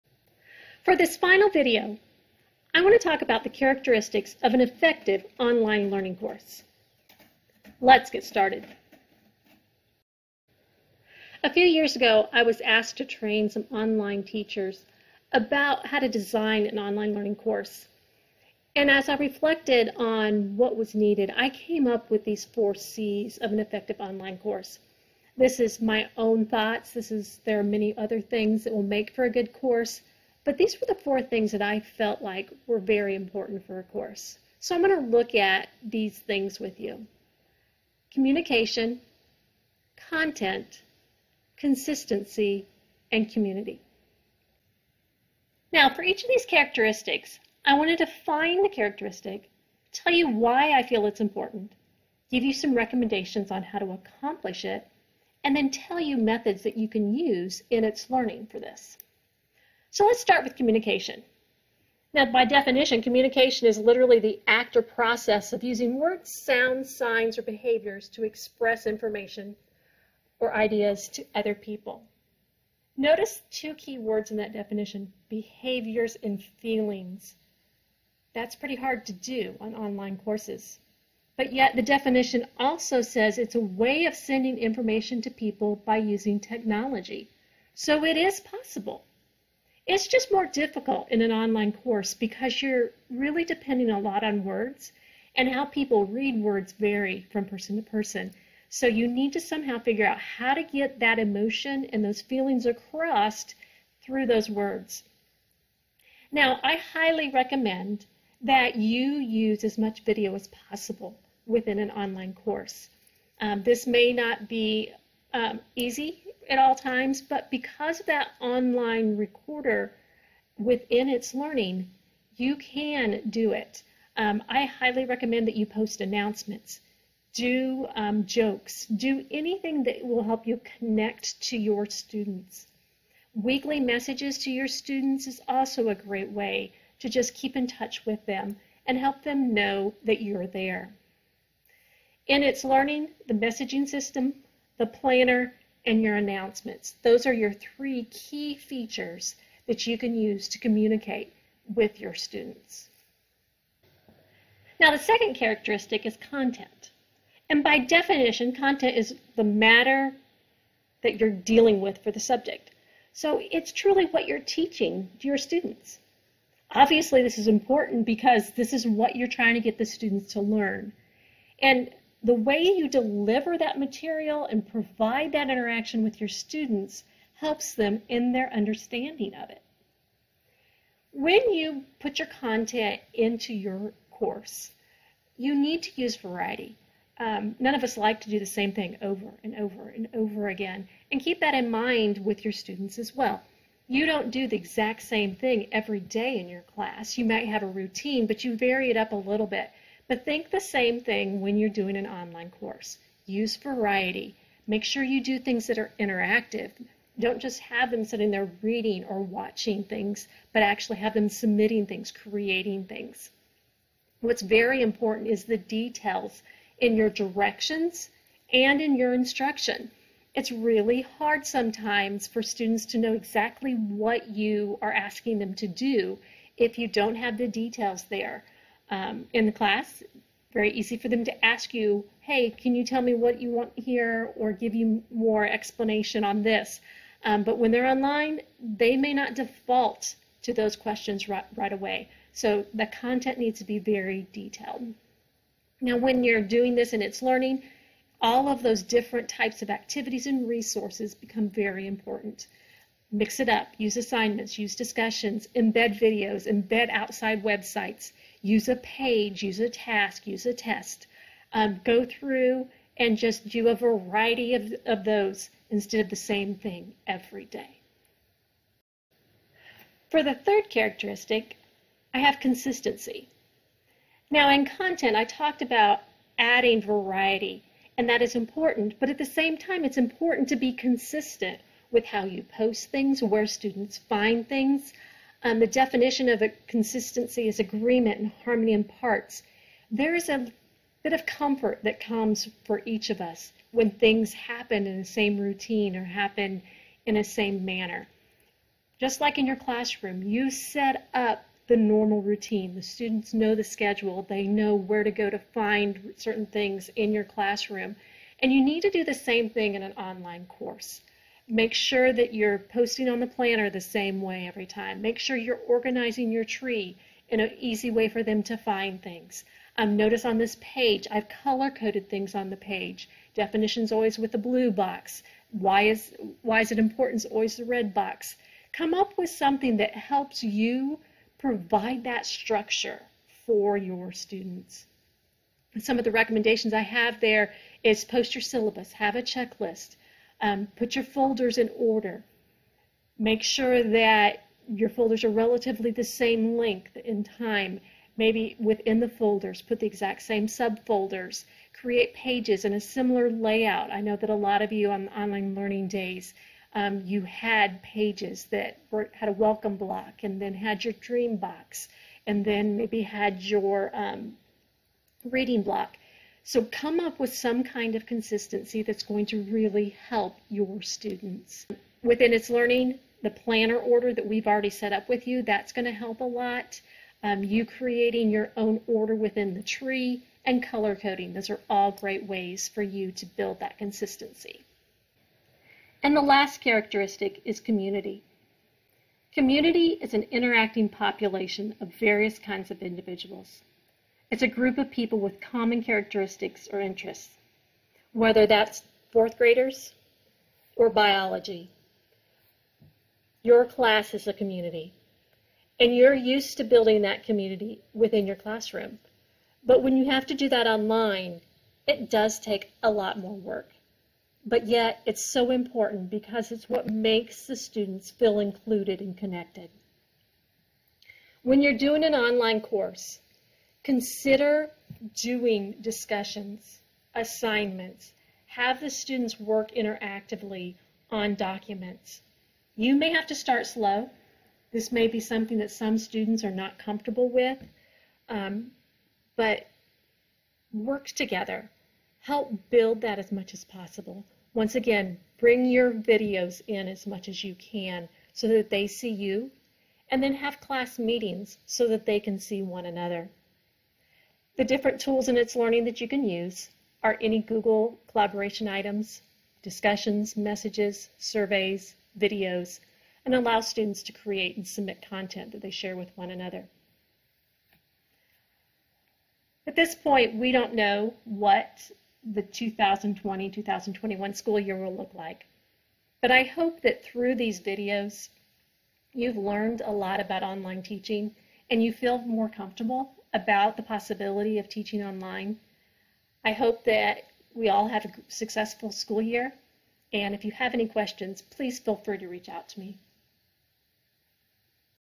Characteristics of Effective Online Learning Course
This video discusses four characteristics of an effective online learning course:  communication, content, consistency, and community.